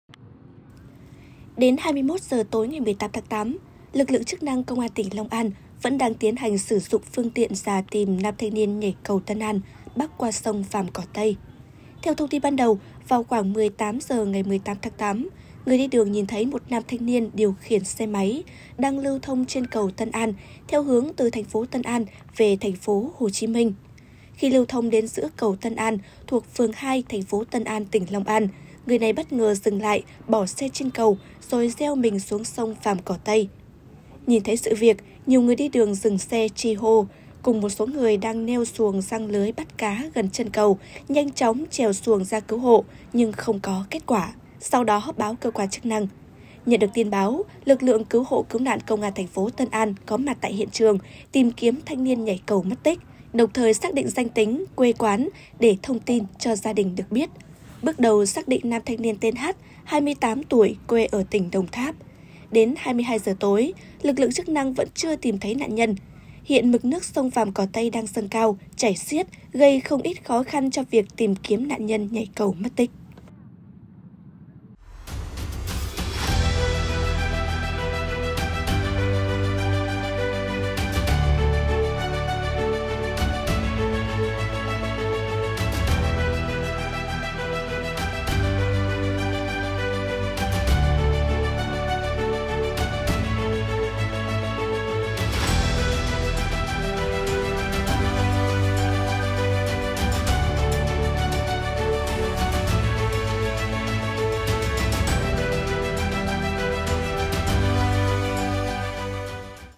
Long An: Nam thanh niên nhảy cầu Tân An mất tích - PLO
Nam thanh niên chạy xe máy, đến giữa cầu Tân An thì dừng xe, gieo mình xuống sông Vàm Cỏ Tây mất tích.